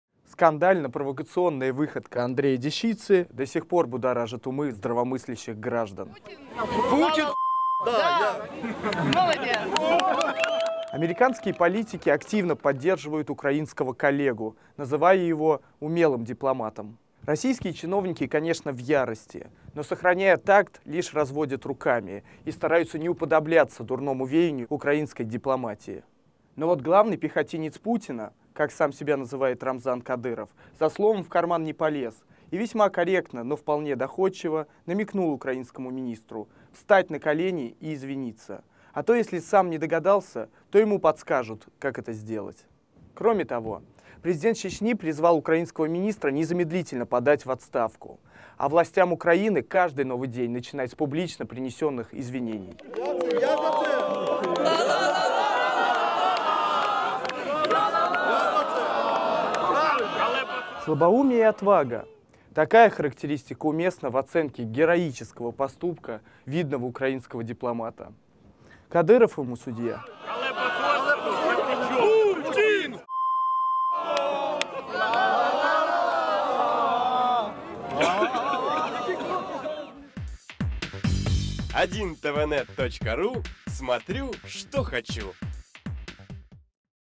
Кадыров объяснил главе украинского МИДа, как просить прощения за оскорбление Путина
Глава Чечни Рамзан Кадыров заявил, что исполняющий обязанности министра иностранных дел Украины Андрей Дещица и все представители киевской власти должны понеси наказание за нецензурные высказывания политика в адрес президента России Владимира Путина.

«Украинские чиновники не знают, что такое государственное строительство, демократия, сотрудничество. Подобные нецензурные высказывания не позволяют себе даже должностные лица воюющих стран. Я твердо убежден в том, что власти этой страны каждое утро должны начинать с публичных извинений за свой хамский поступок. Дешица оскорбил весь российский народ, всю Великую Россию. За это придется ответить», — написал Кадыров в своем Instagram.

Глава Чечни отметил, что украинский министр не «отделается детским лепетом, что подпевал толпе и больше не будет».

«Можете быть уверены, что есть кому спросить не только за то, что публично сказано в его адрес, но и за то, что Дешица, стоя на коленях не попросил прошения. Если же Дешица не догадался встать на колени, то ему доходчиво подскажут! Дешица должен немедленно подать в отставку! Другого выхода у него нет», — отметил Рамзан Кадыров.

Напомним, накануне и.о. главы МИД Украины Андрей Дещица, выступая у здания российского посольства в Киеве, позволил себе нецензурные оскорбления в адрес президента Владимира Путина. Дещица заявил, что таким образом он пытался снизить напряженную ситуацию у здания посольства и успокоить толпу.

Его российский коллега Сергей Лавров заявил, что Андрей Дещица воспринимается в России как «отрезанный ломоть».

«Глядя на него, вспоминается хорошее выражение: не умеешь петь, не пей», - сказал глава МИД России Сергей Лавров российским журналистам в понедельник в Белграде.